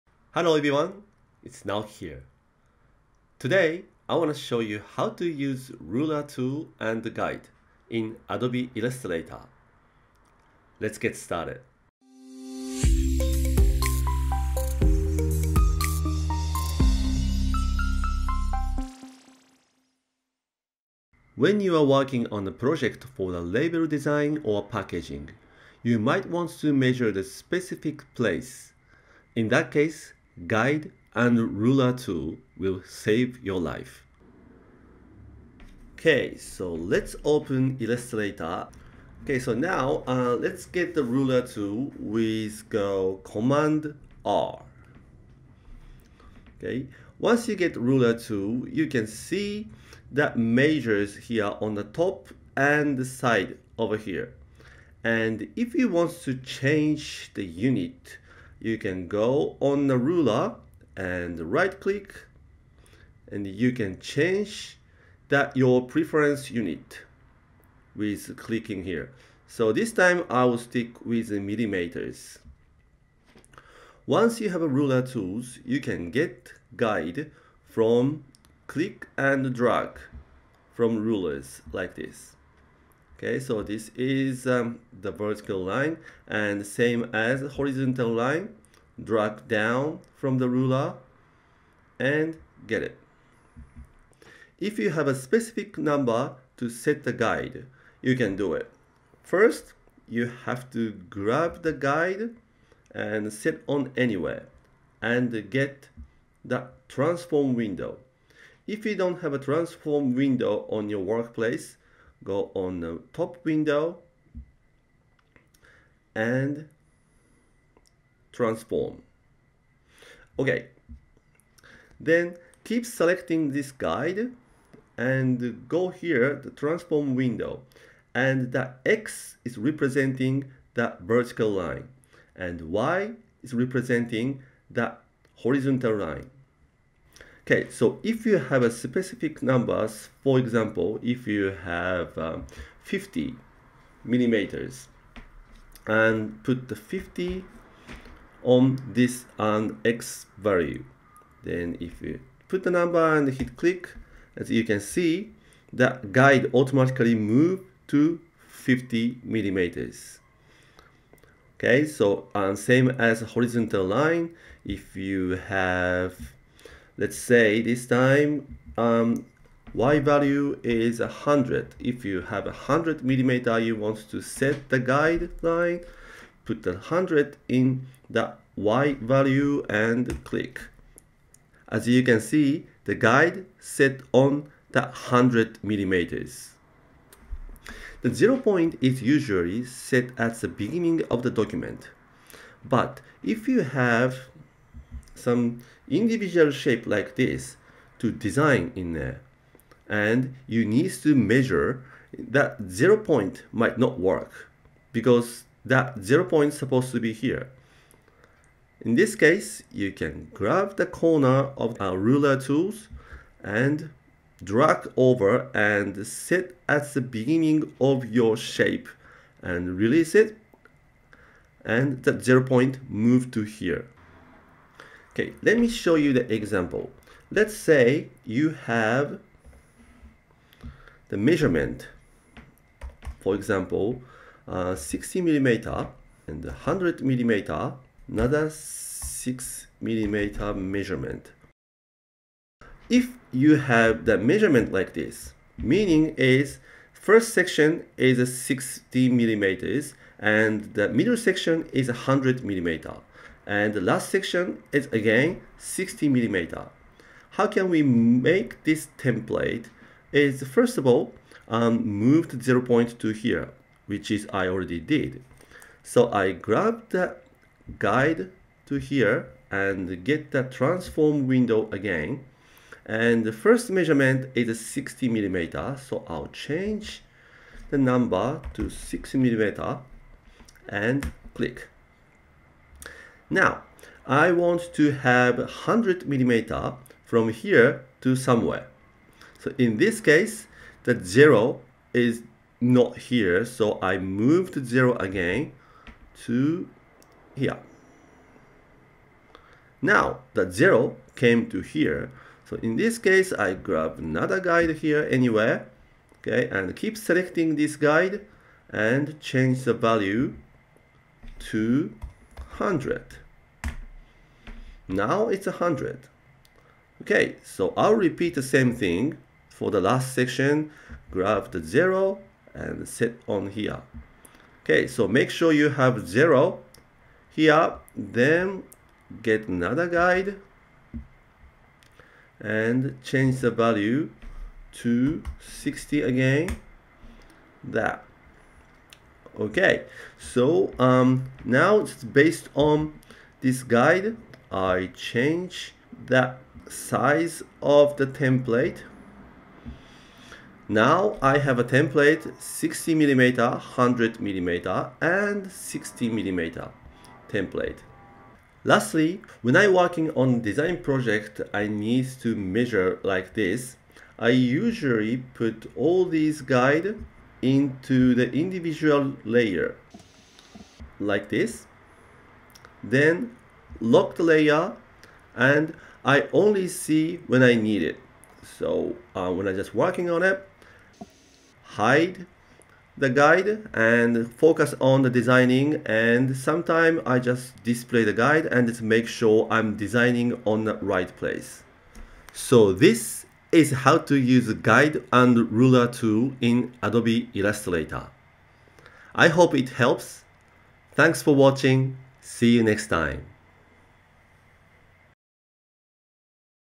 Adobe Illustrator tutorial - How to use Ruler Tool and Guide
Hello everyone, it's Naoki here!

In this video, you will learn how to use Ruler Tool and Guide in Adobe Illustrator for beginners. I hope you enjoy it :)